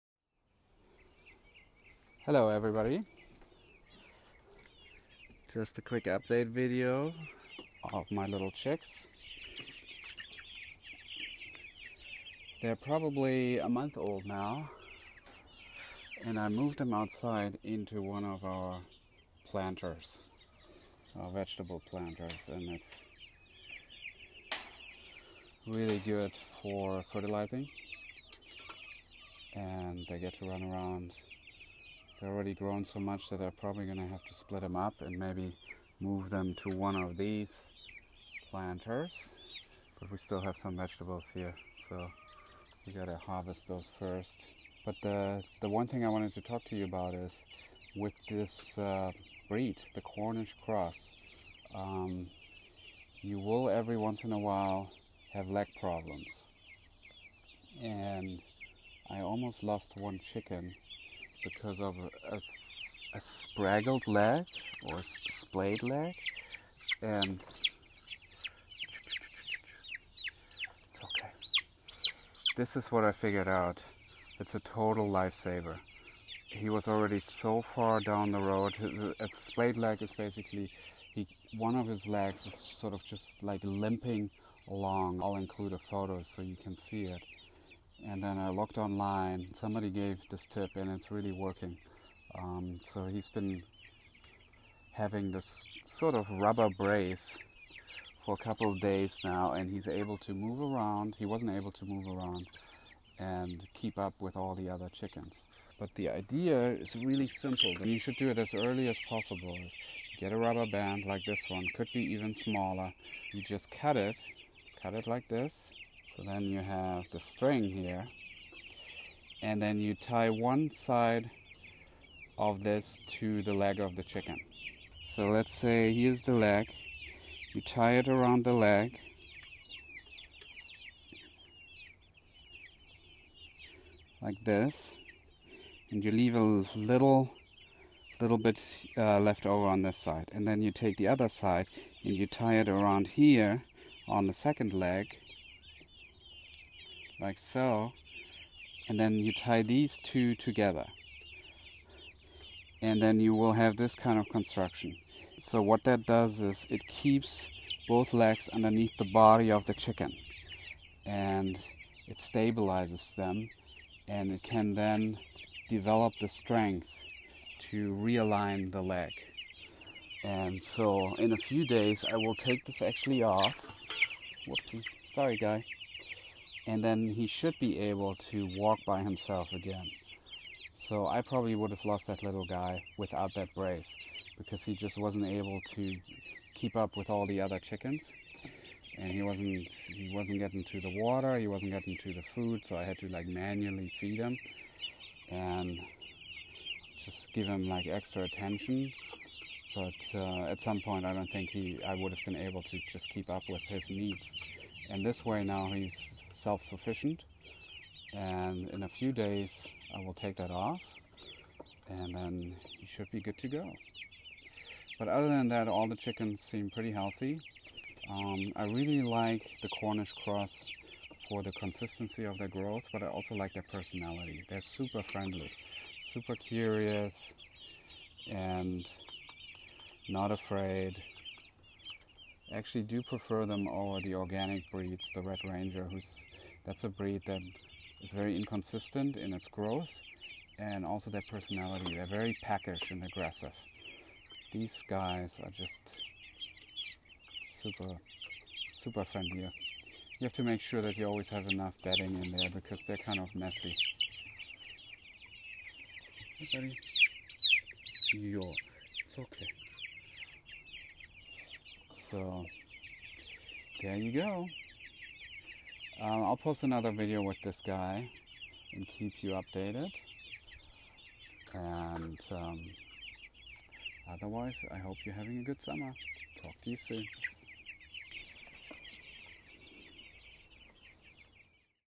Splayed Legs - Treatment of a Spraddle Leg (Slipped Tendon) of a Chicken
Use a very thin rubber band (the very thinnest kind).  Cut it so you have a long stretchy string.  Then tie one end around one ankle and leave a long piece/end sticking out.  Then tie the other ankle with a loose knot leaving about an inch between legs so that the feet stay under the body.  After you tie a loose knot around the second ankle you then tie that end of the "string" to the tied off other end of the string.  So under the body the string is tied to istelf.  That way as the chick runs and walks around they don't manage to pull the rubber band knots too tight on their ankles. If you don't tie the ends of the string to the other end, the chick will walk and it will pull the rubber band and possibly tighten the knots around the ankle.

With this method the chick can learn to walk.  It gives them mobility but keeps their feet under their body.  After about a day, they will be running and walking with it like all the rest.  Check the knots often just to be sure they don't pull tight on their ankles and leave it on for about a week.

Supposedly works every time.